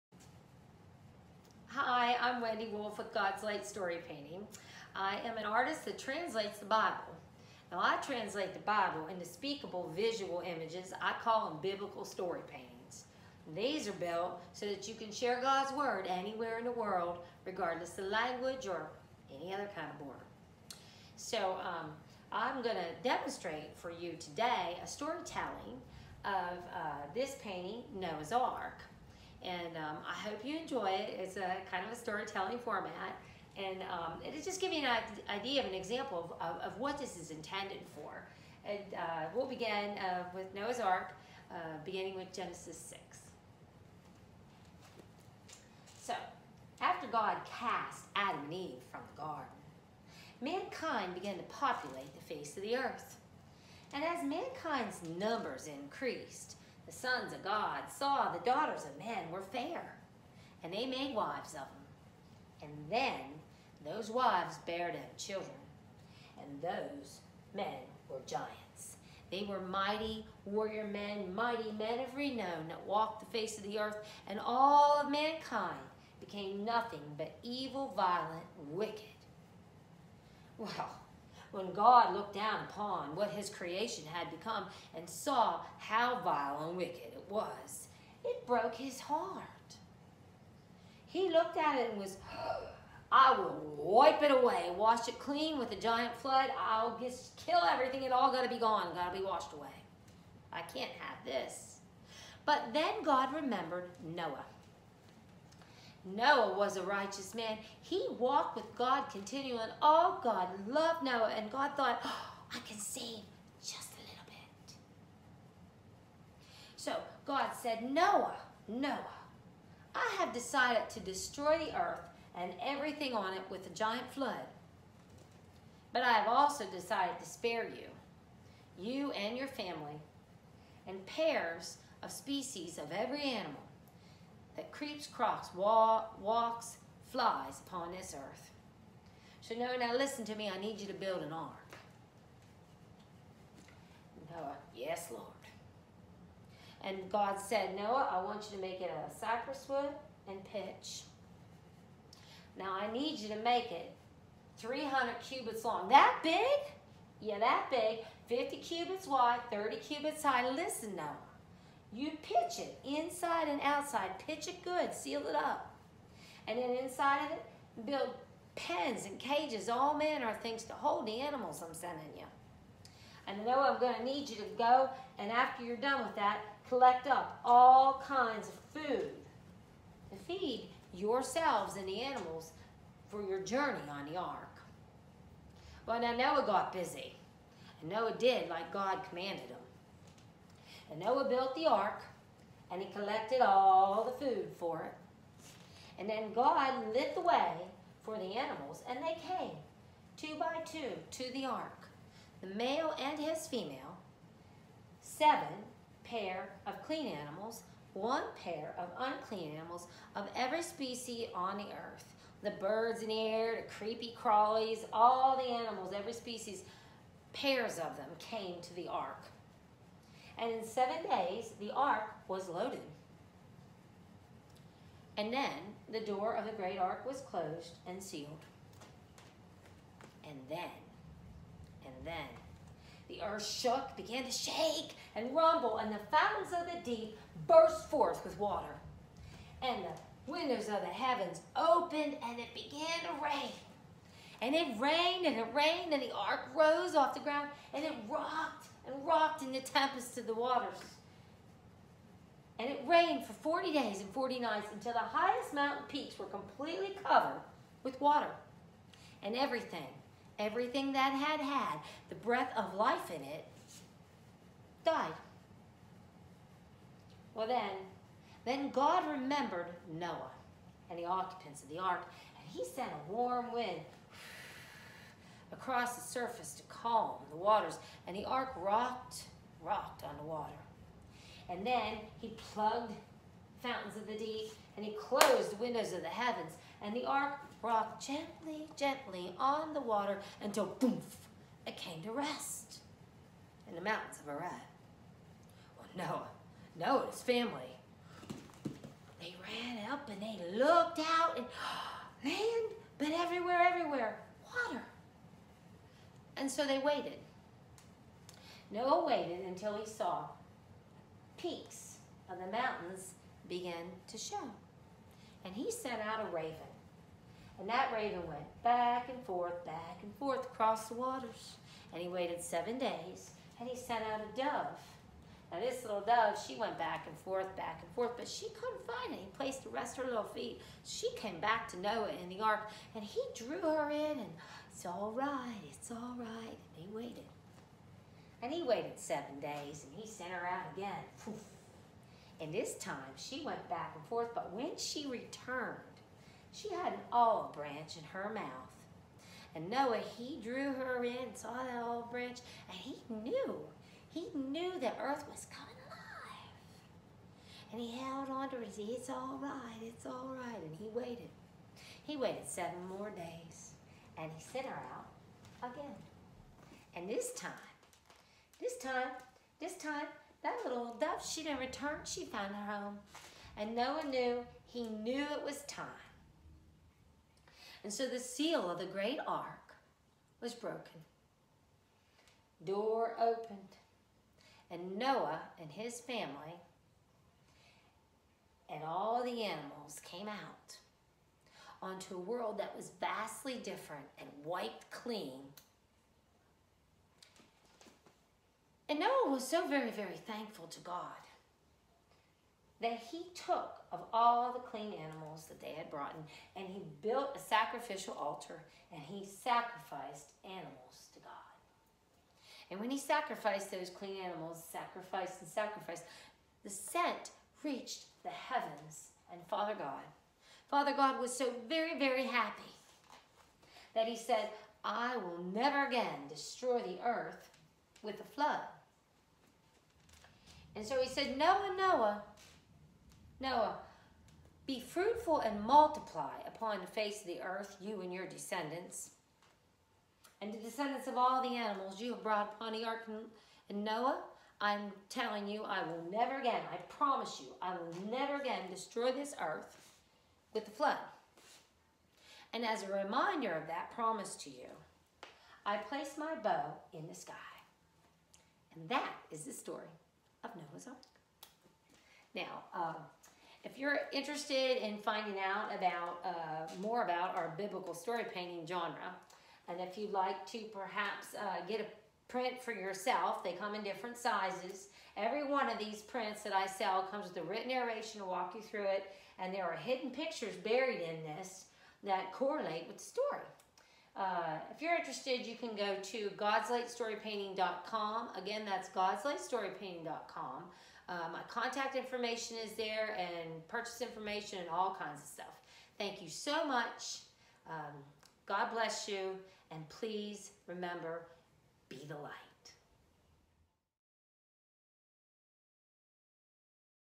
"Noah's Ark" A Biblical storytelling performance by Artist/Storypainter, Wendy L Wolf
Our artist translates the stories of The Bible into speakable visual images. This unique Storytelling Art allows you to share Bible Stories across borders, in any language.
This is a reading of our "Noah's Ark"  Biblical Storypainting.